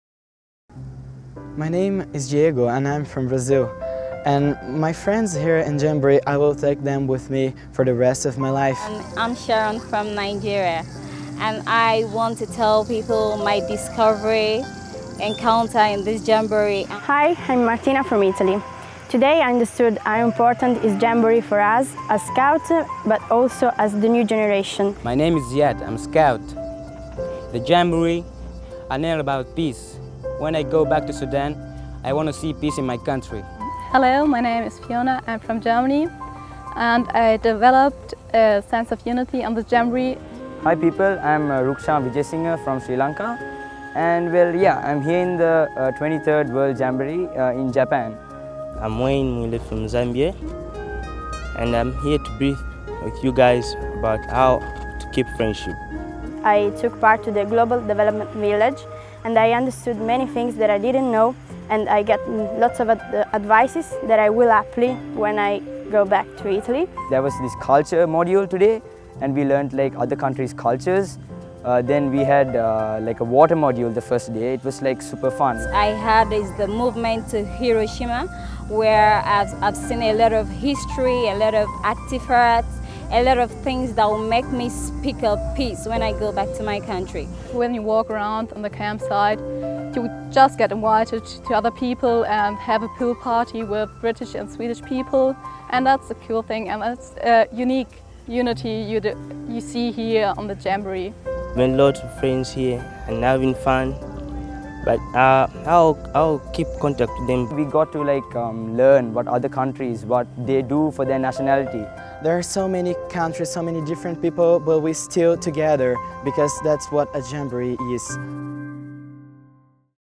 Closing Ceremony Participant Testimonials
Watch touching testimonials of how the Jamboree has changed the lives of these scouts and how it will help them work to make this a better world